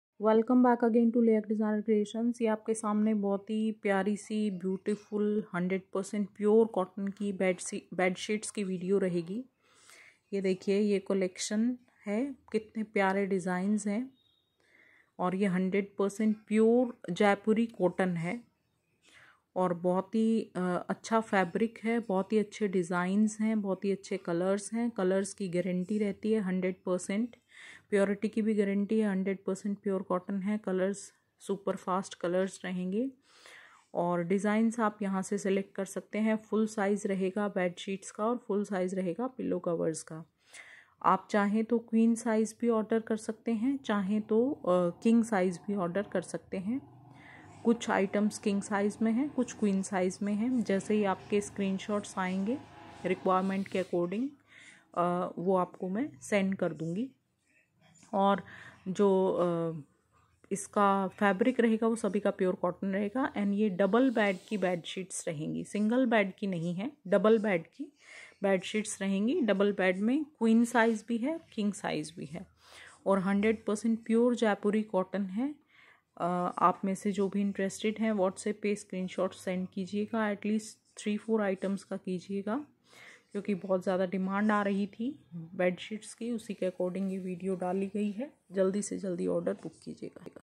Beautiful Pure Cotton handblock Print Double Bedsheets with pillow covers@lehakdesignercreations8940
Beautifully Crafted Pure Cotton hand block Print Double Bedsheets collection with 2 pillow covers
                                                                                                                                                                                                                 *Crafted with Beautiful Handblock Prints*

.💯% Natural Dye🌿
💯%  Hand Crafted💫
💯% Color Fast ✔️
💯% No Shrinkage✔️